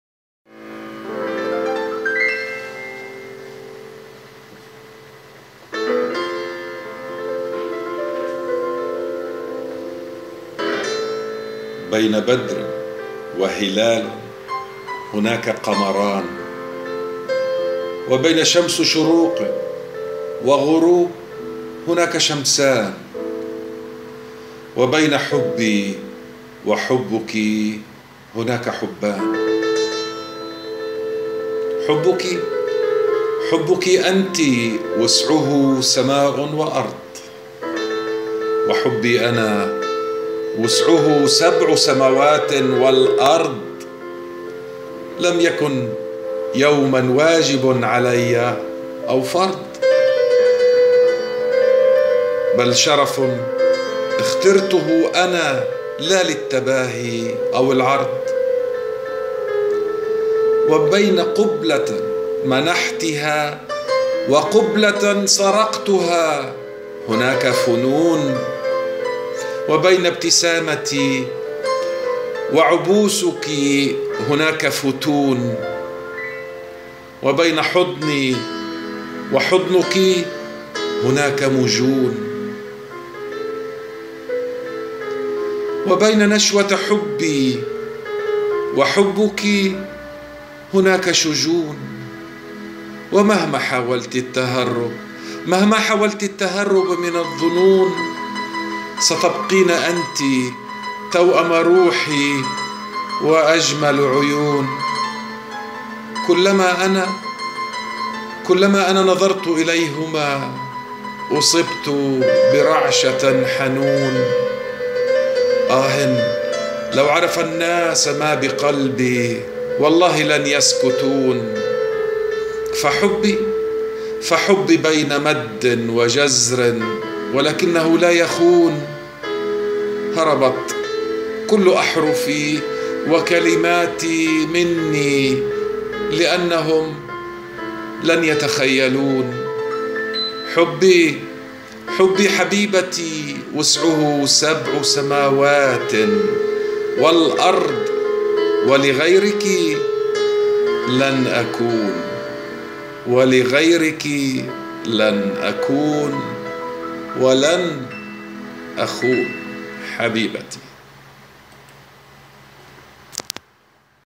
Dr. Ahmed Budeiri د. أحمد البديري - من قصيدة أنت القمر و أنت حبي
قصيدة : أنت القمر و أنت حبي .. من ديوان رحلة مع الحب للشاعر د. أحمـد البديري
بين بدر و هلال هنلك قمران .